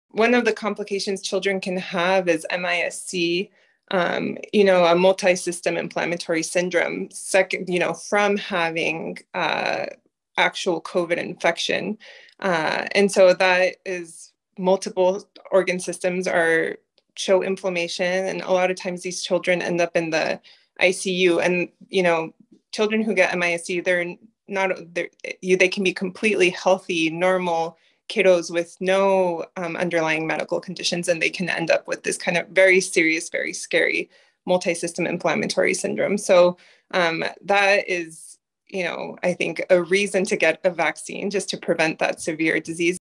Let's Talk Kids and COVID-19: Multisystem inflammatory syndrome (MIS-C)
Multisystem inflammatory syndrome (MIS-C) is a rare but serious condition that may develop in children and adolescents who have had COVID-19. Dr. Maria Fernandez, family medicine physician @CommunityHealthAllianceNevada, shares why it's important for all kids to get vaccinated.